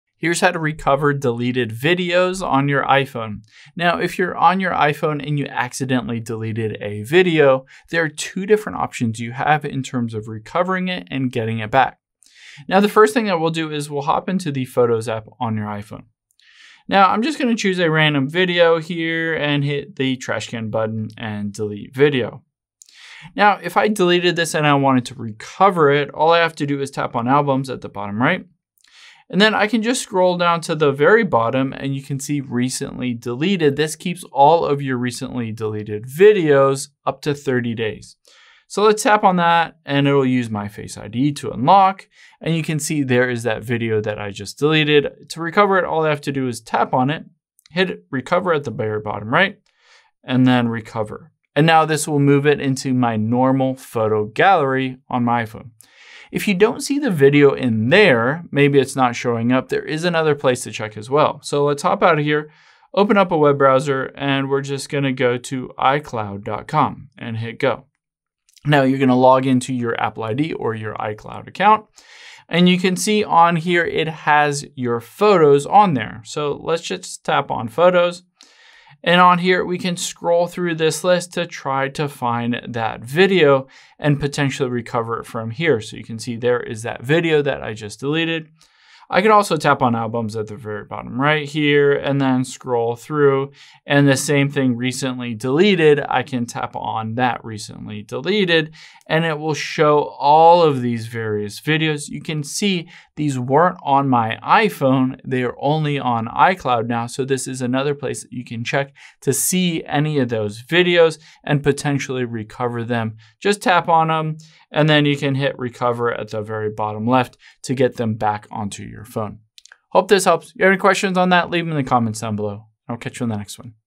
How To Recover Deleted Videos On iPhone
Let's go over how you can recover any deleted videos on your iPhone if you had a video, deleted it and want to get it back.